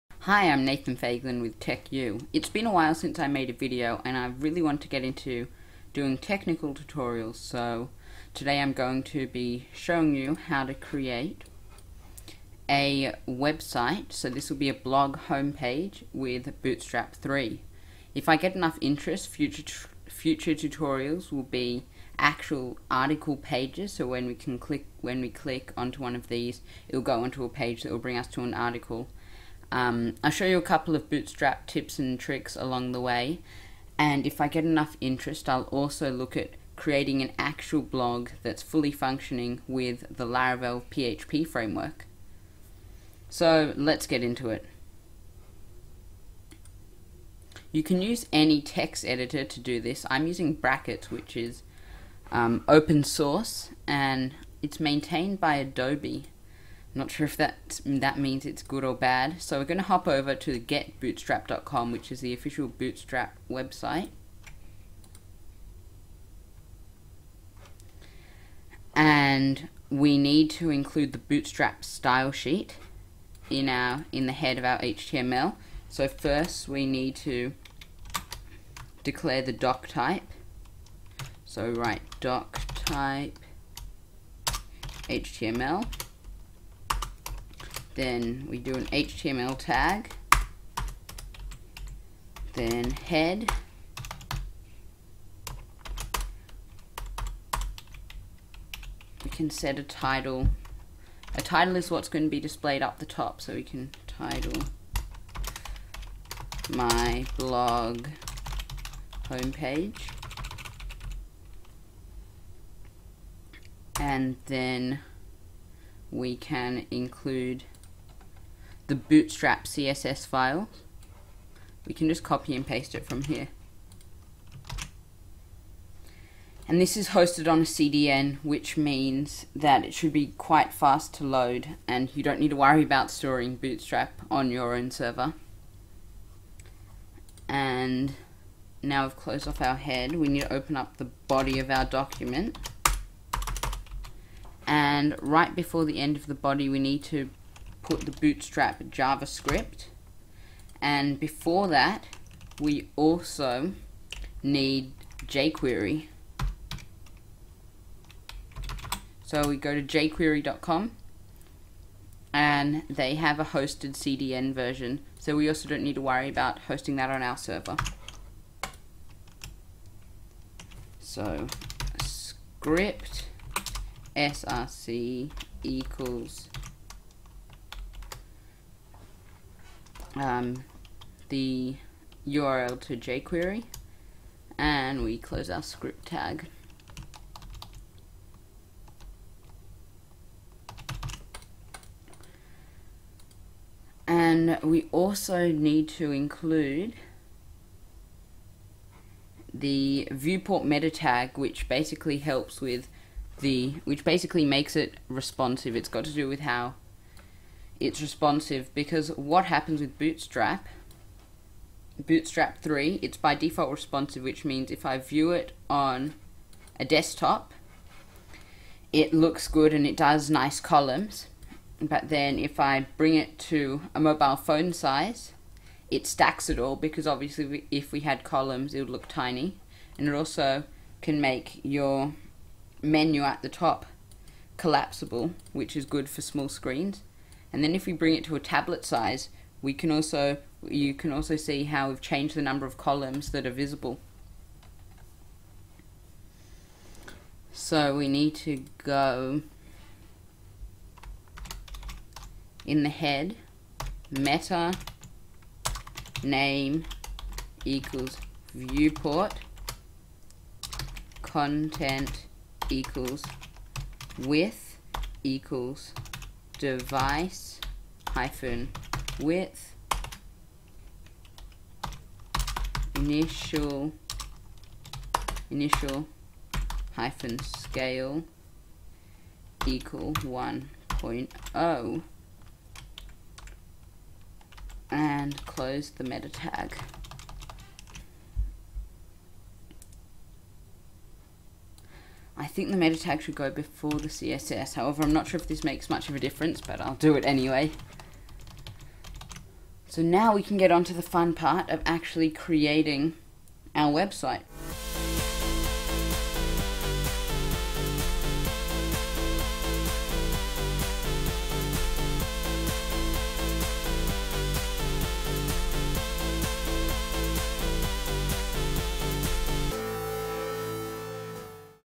Bootstrap 3 Tutorials - Create a responsive website with Bootstrap 3
In this tutorial, we get started by including Bootstrap 3 in our file to get us set up for the series of Bootstrap tutorials where we will create a responsive website.